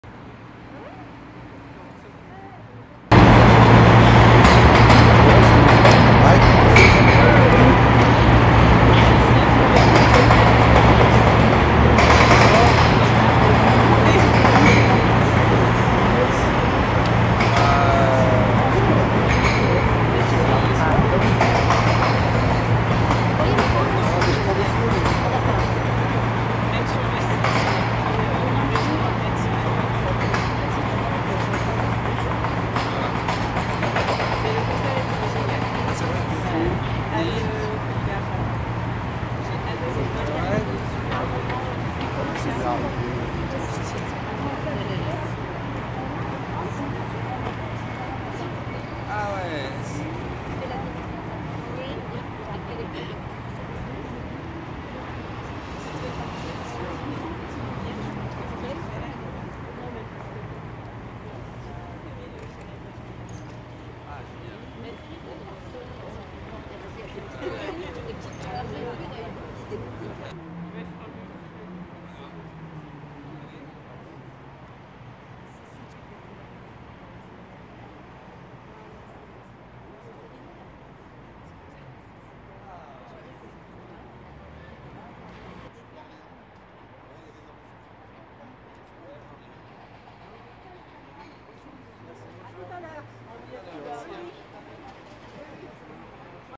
EXCLUSIVE : Melvil Poupaud arriving at RTL radio station in Paris
Melvil Poupaud arriving at RTL radio station in Paris

Paris, France 19th November 2018